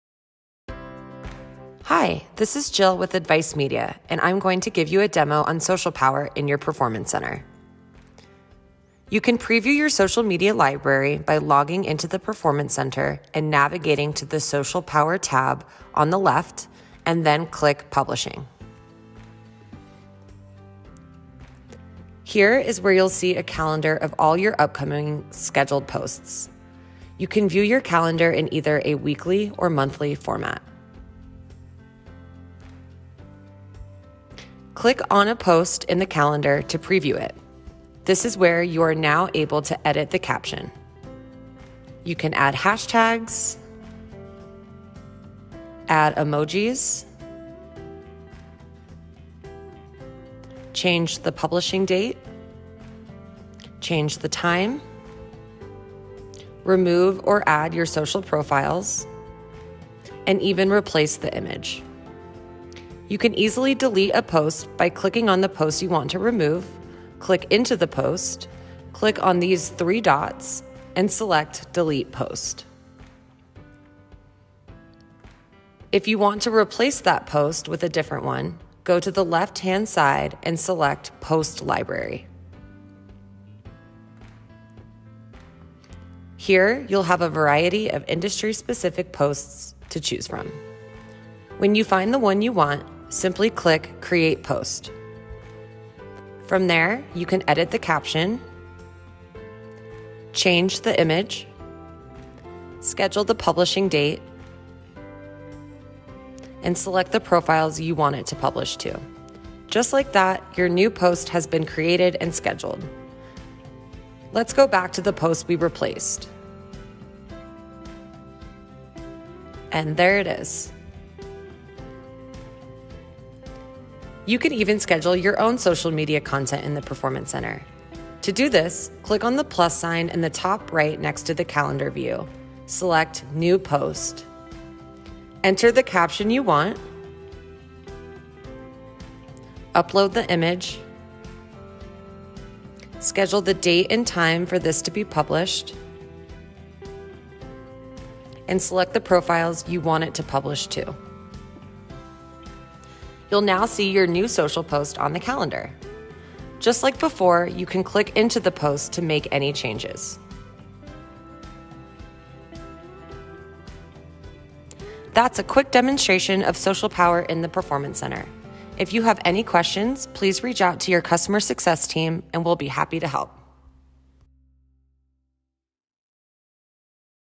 Social Power in the Performance Center | Advice Media
Advice Media's Social Power tool is now in the Performance Center! View this demo to see how you can view your upcoming posts, see your post library, edit your posts, and schedule your own content.

Learn how Advice Media will get you more clients & receive a free website evaluation